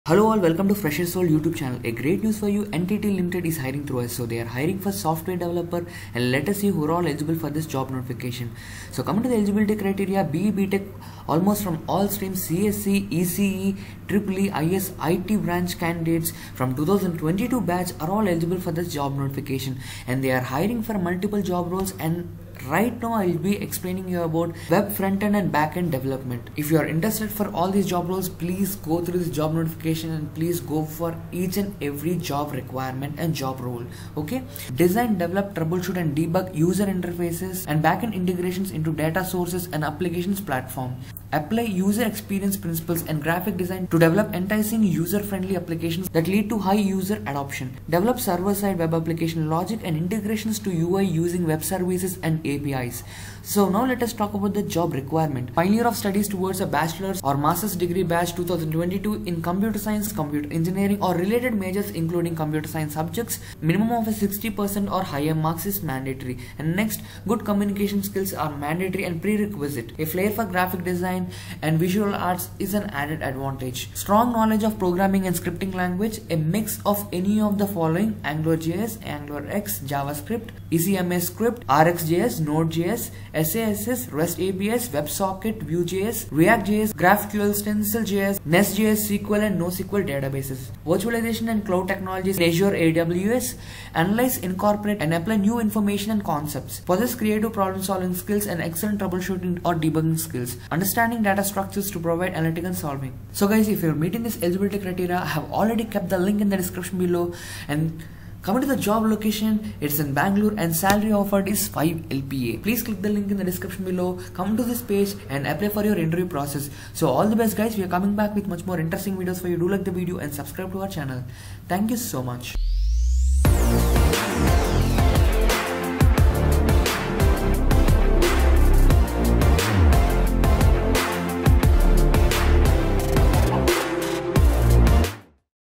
NTT Ltd Hiring for Software Developer | 5 LPA, Apply Now
Test to get hired in the top companies; Relevel.com Register now

Job Role:
Design, develop, troubleshoot and debug User Interfaces (web portals, mobile applications) and back-end integrations into data sources and applications platforms
Apply User Experience (UX) principles and graphic design to develop enticing, user-friendly applications that lead to high user adoption
Develop server-side web application logic and integrations to UI, using web services and APIs
Perform production support of solutions developed to ensure consistency of business delivery